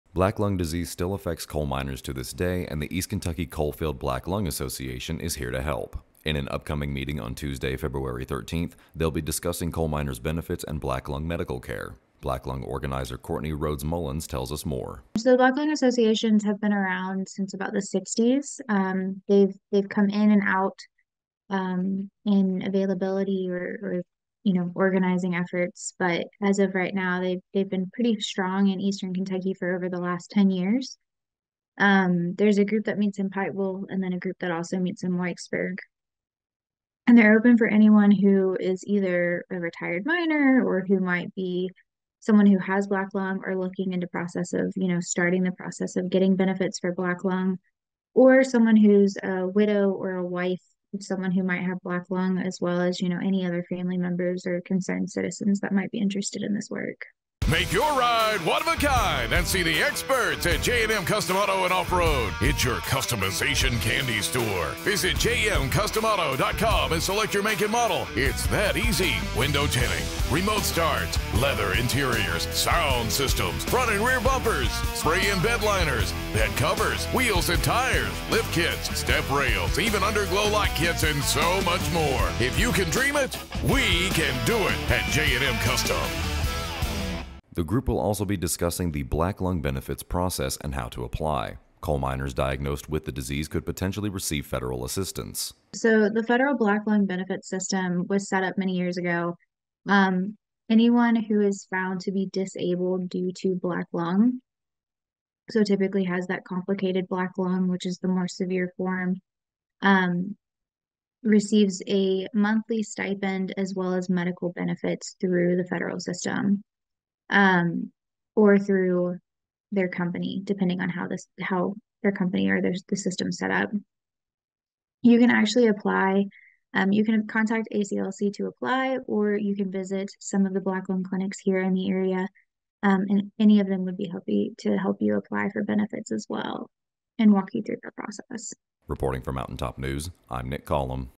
East Kentucky Coal Field Black Lung Association to Hold Meeting to Discuss Benefits and Care
East Kentucky Coal Field Black Lung Association will be holding a meeting to discuss benefits for coal miners and black lung medical care on Tuesday, February 13, 2024.